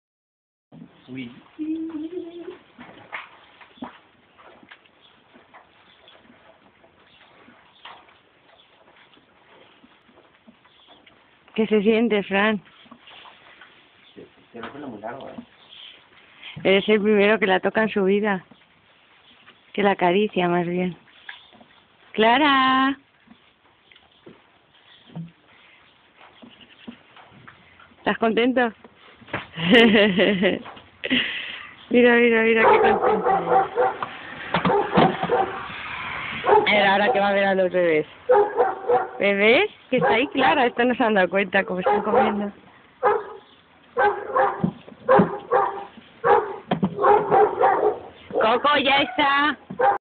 Clara, su primera caricia
La bebé del Santuario-escuela, Clara, hoy día 04 ha tenido su primera caricia.
¿Os imagináis?, tiene 4 meses y nunca ha tenido un contacto placentero con humanos. La separaron de su madre cuando apenas contaba con 1,5 meses, y la mantuvieron encerrada hasta los 4 que tiene ahora. Su relación con nuestra especie siempre ha sido brusca, con miedo, y hoy...toda su vida empieza a cambiar, nunca más miedo, nunca más sola, ya es parte de El Hogar.